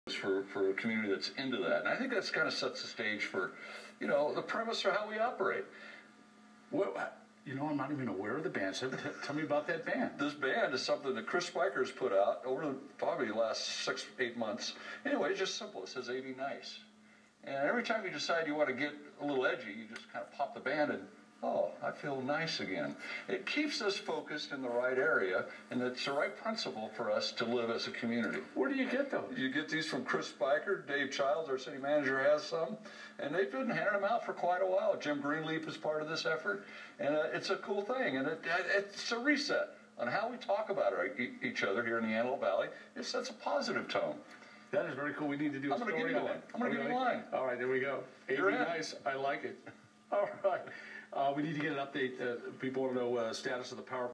Mayor Ledford, Mitchell Chase TWC AVNice interview Part Deux
Video uploaded from my mobile phone. Great picture, sound quality. AVNice, a program to bring people together, to create conversation and harmony seems to be getting some traction. Life is not simple, it can be complex, but we all live in one region, and IF we can talk, we can accomplish so much more.